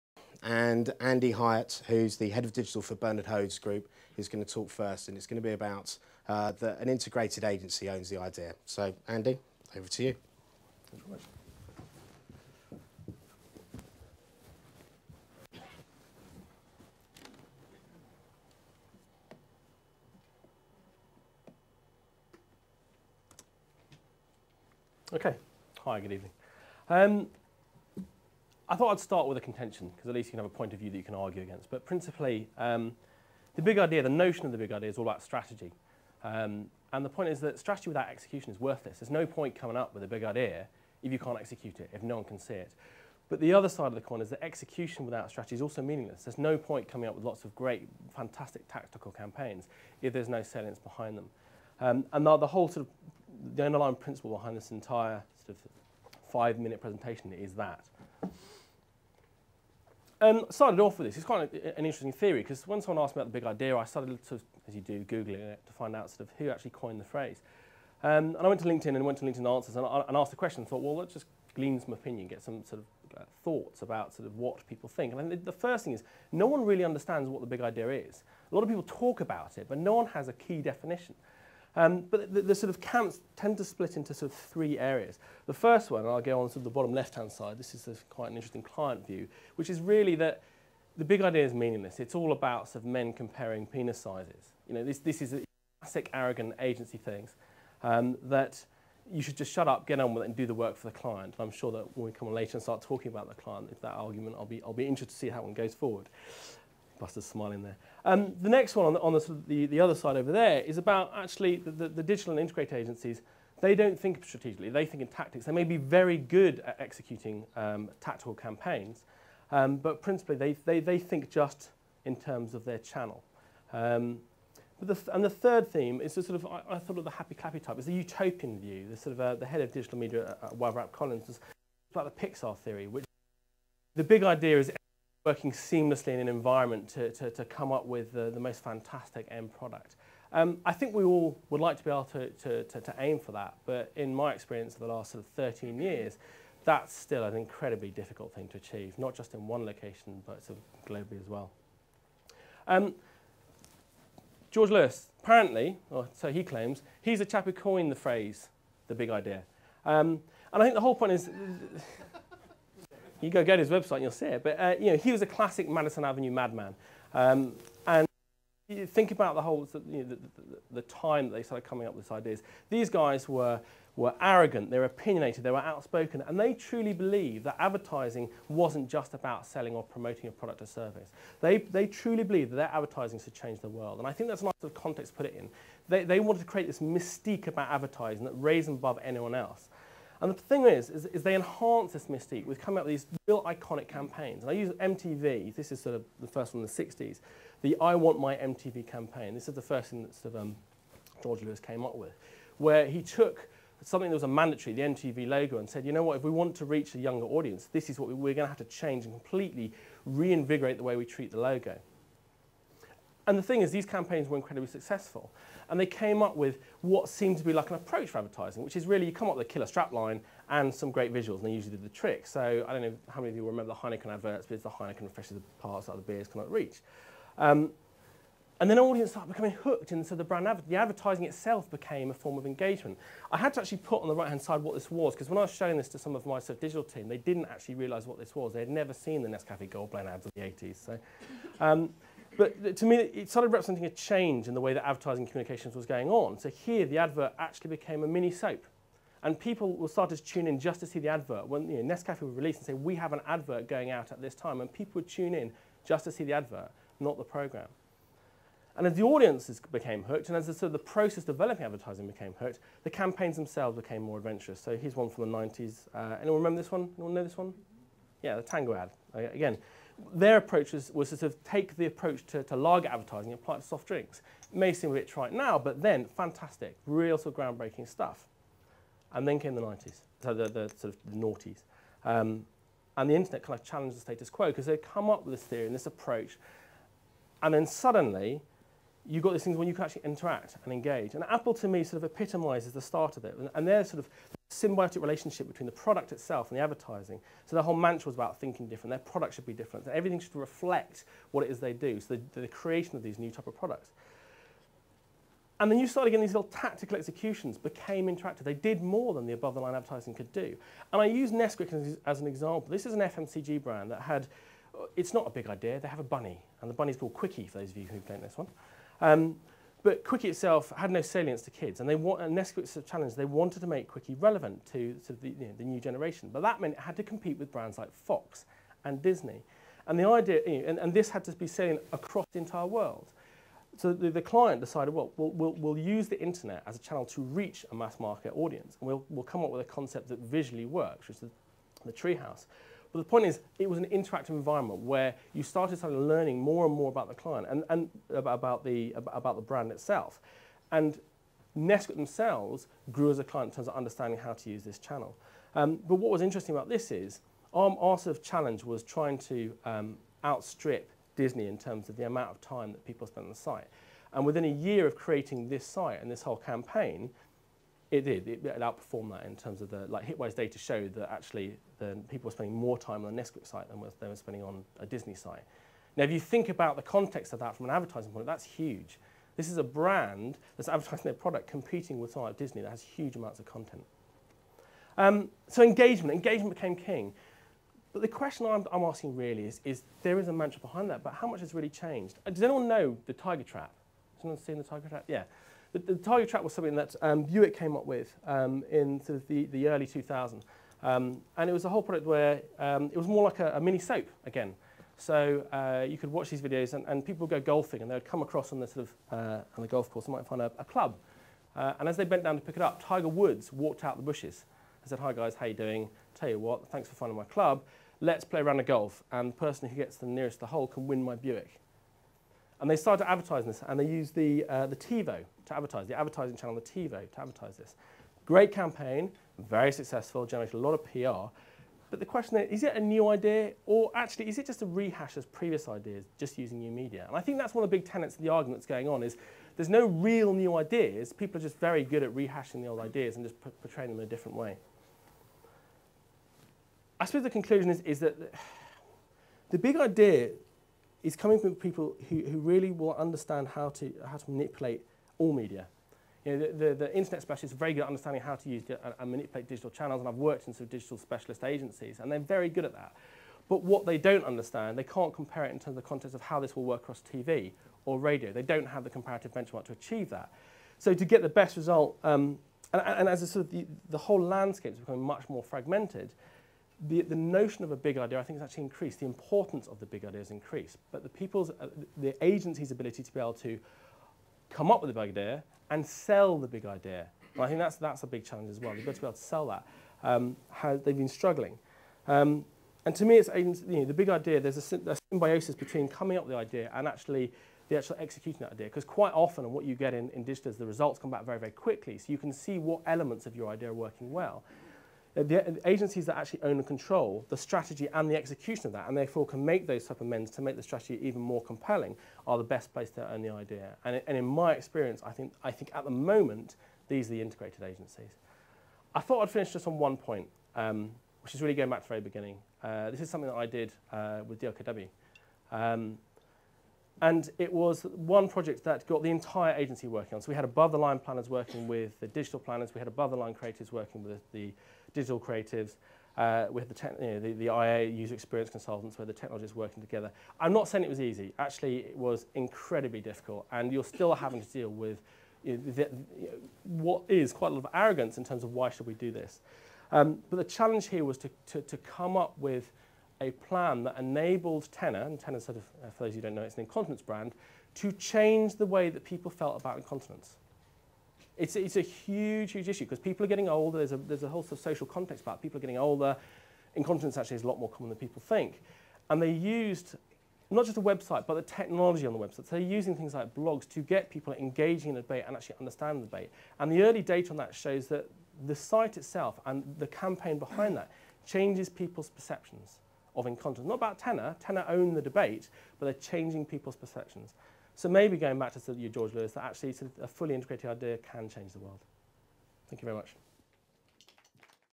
Integrated agencies best placed to own the 'big idea'
Blue Skies Marketing Recruitment seminar - Andy Hyatt, Head of Digital at Bernard Hodes Group, puts forward his stand point that integrated agencies are best placed to own the 'big idea'